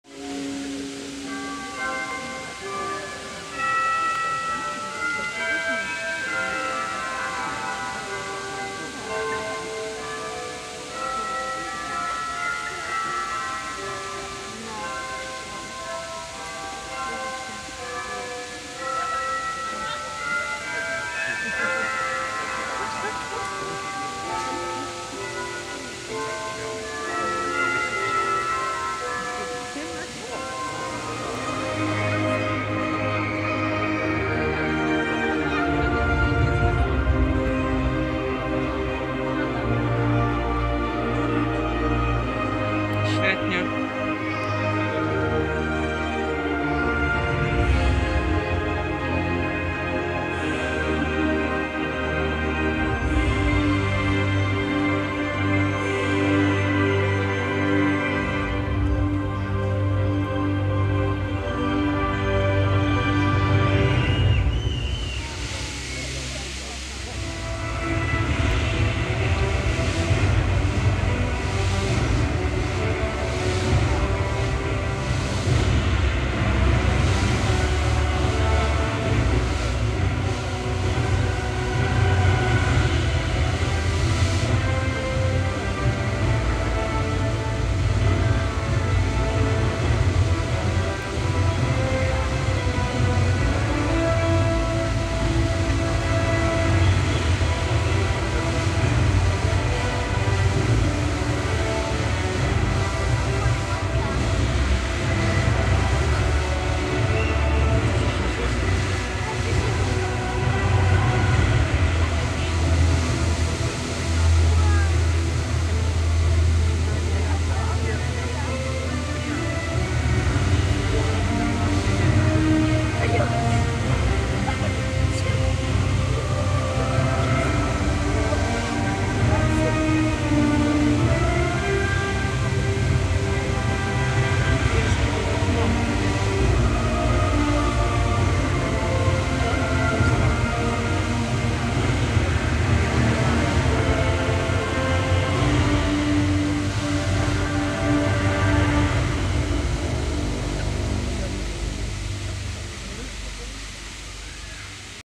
Fontanna Multimedialna w Rzeszowie - nagranie amatorskie
Nagranie telefonem komórkowym. Fontanna Multimedialna w Rzeszowie. W rzeczywistości jest to niesamowity pokaz, który jest jedyny w swoim rodzaju. Efekty wizualne są niesamowite - połączenie muzyki i światła, a wszystko to wyświetlane jest na tafli wody fontanny.